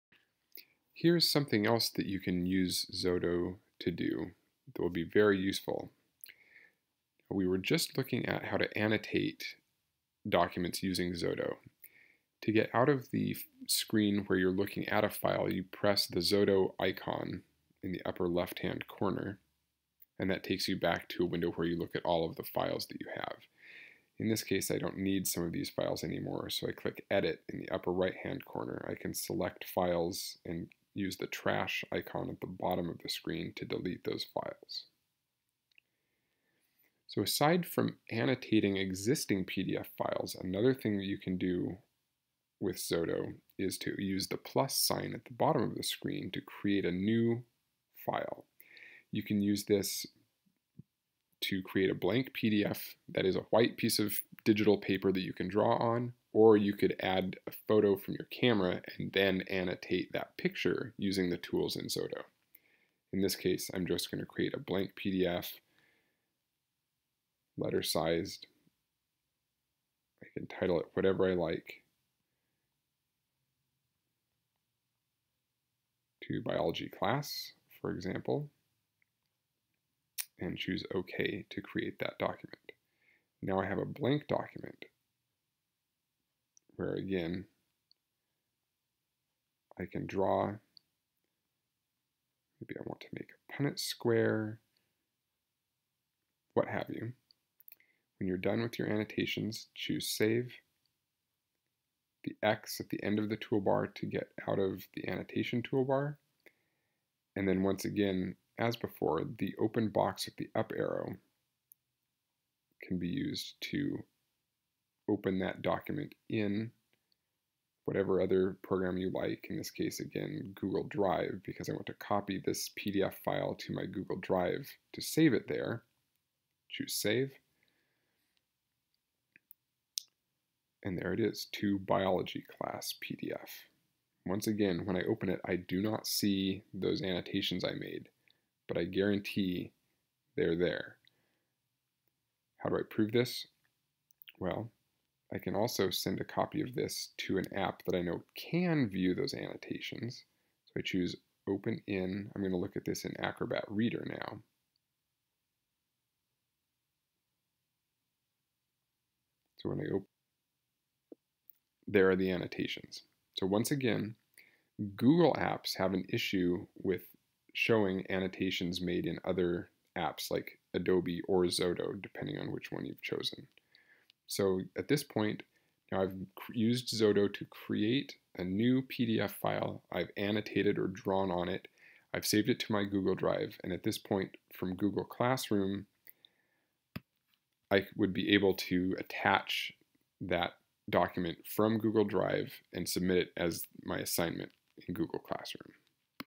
Creating a new PDF in Xodo
Screencast of a process we will use quite frequently in class:
1) creating a blank PDF file in a PDF annotation program (Xodo)
2) Using Xodo to annotate the PDF
3) Exporting the annotated PDF from Xodo back to a Google app (Drive)
With an annotated PDF saved to your Google Drive account, it is easy to then attach that file to your Google Classroom assignment for submission